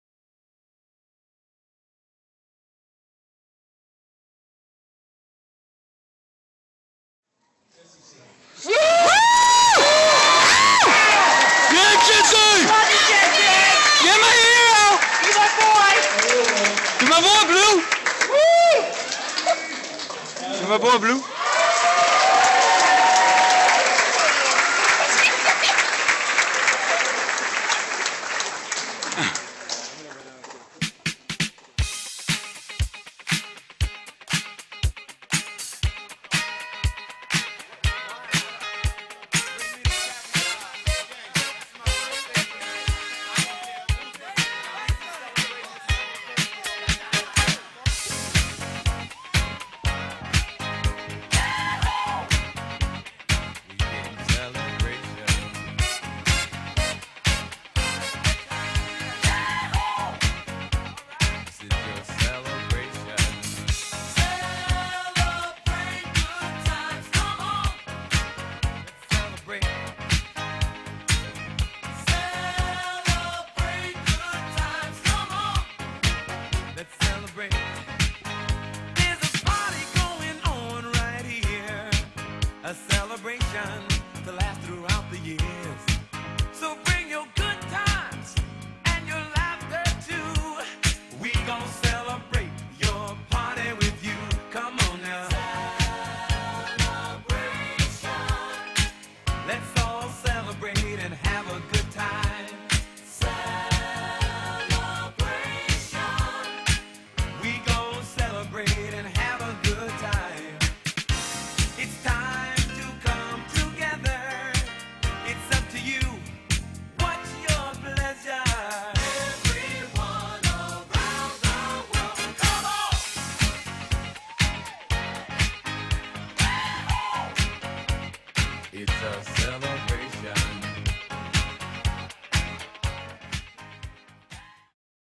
grad day 2012 at project forward
this is for all my friends who had there grad day at cape cod cc